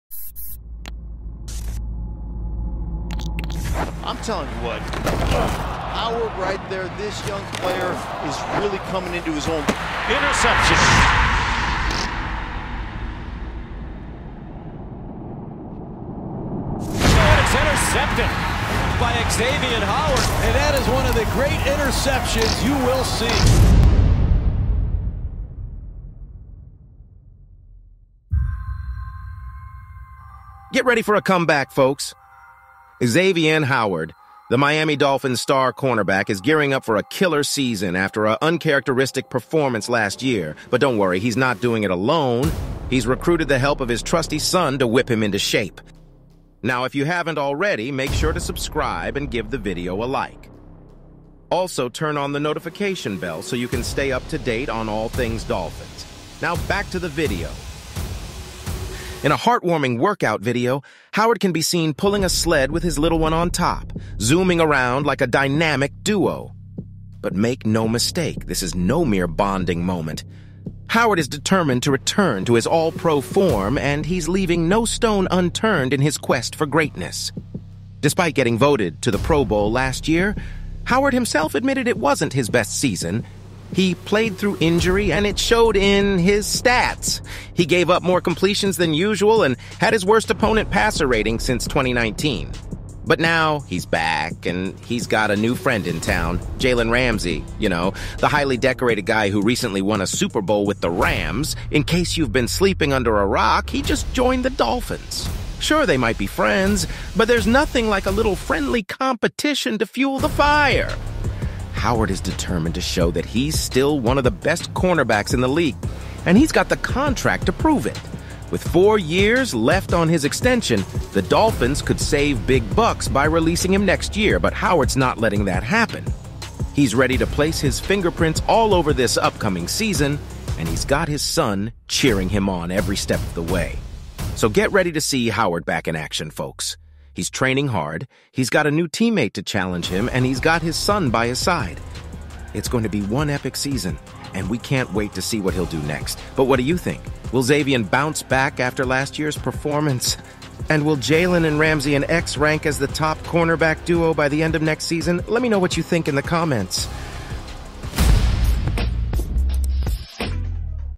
🔥👀 MOTIVATED Xavien Howard Putting in WORK for COMEBACK Season!! Miami Dolphins Highlights
Written and Produced by Joey Tejedo and Kevin Tejedo